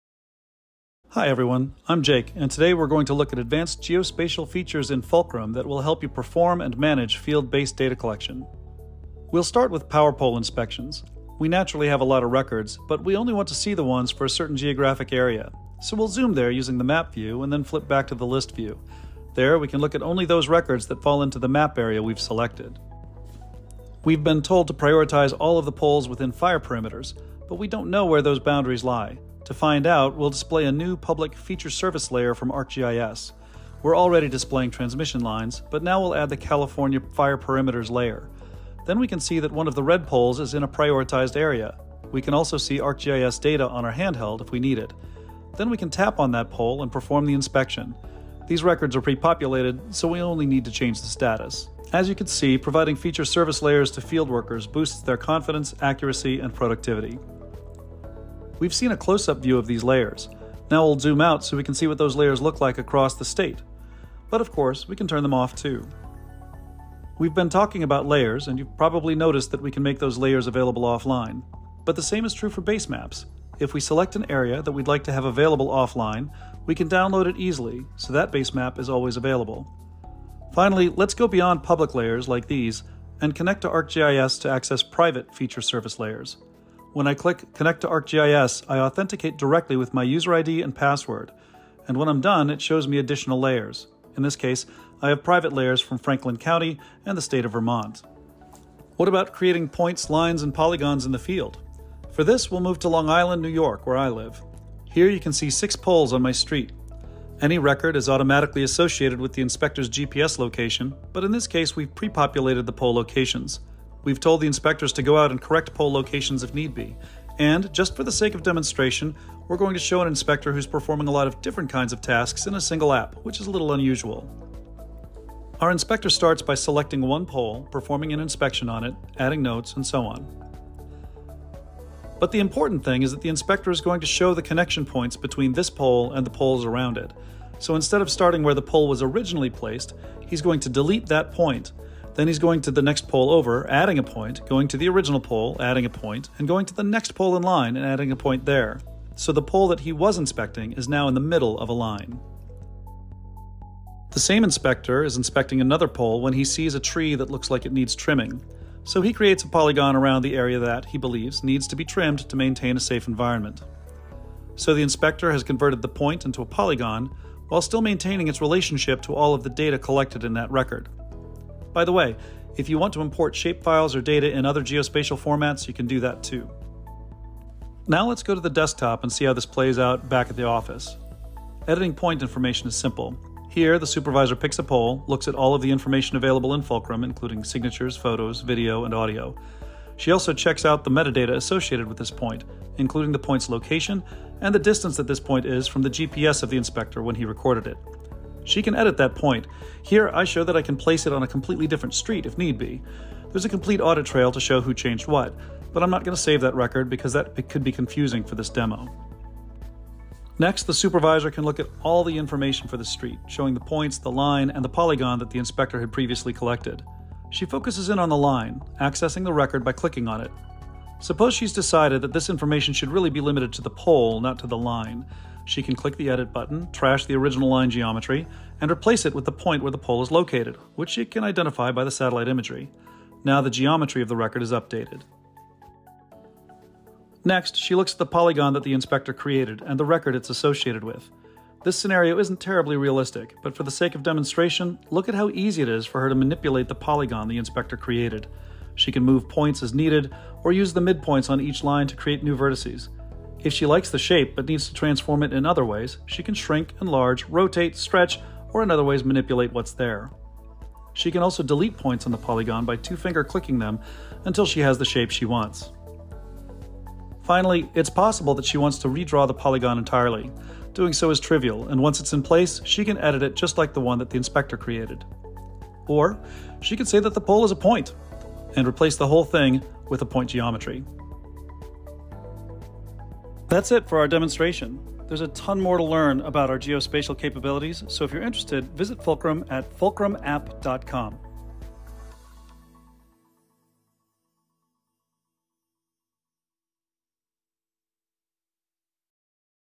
Fulcrum GIS Lines and Polygons for Power Pole Inspections
In this demonstration, Jake introduces advanced geospatial features tailored for field-based data collection. He focuses on power pole inspections within specific geographic areas using ArcGIS's public Feature Service Layer, including the California Fire Perimeters to prioritize inspections. This demo emphasizes the significance of providing field workers with access to feature service layers and base maps, both online and offline, to enhance their efficiency, accuracy, and confidence.

Jake also explores the practical application of these tools in the field, such as creating and editing points, lines, and polygons to accurately reflect field conditions and inspector findings. The presentation highlights the seamless integration between field data collection and back-office editing and management, showcasing the flexibility and power of geospatial technology in managing infrastructure inspections and maintenance.

Engage with Fulcrum on Social Media!